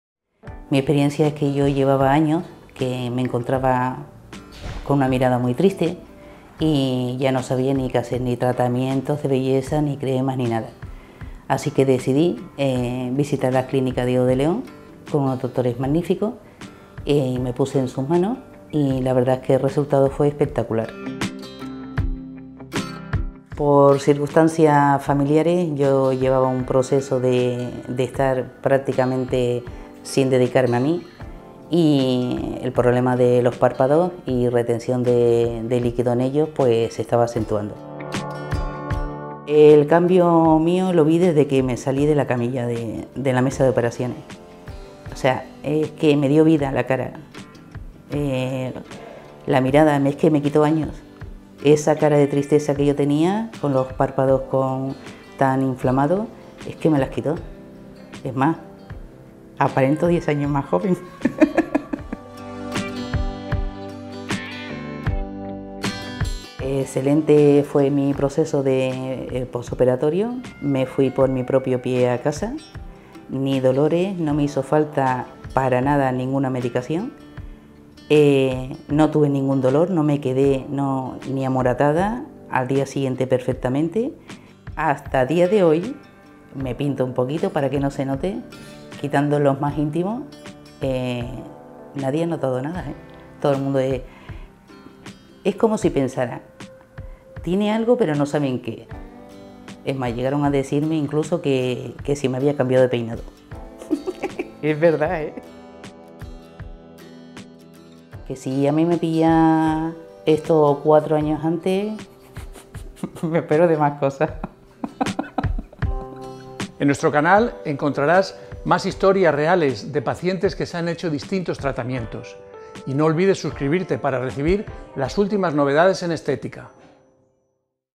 Cirugía para PÁRPADOS HINCHADOS y CAÍDOS superiores | Resultado definitivo con la BLEFAROPLASTIA
Carmen tenía la mirada triste hasta que decidió someterse al tratamiento de blefaroplastia. Con esta cirugía eliminó la piel sobrante de sus párpados hinchados superiores. Un resultado definitivo con el que ha rejuvenecido su mirada.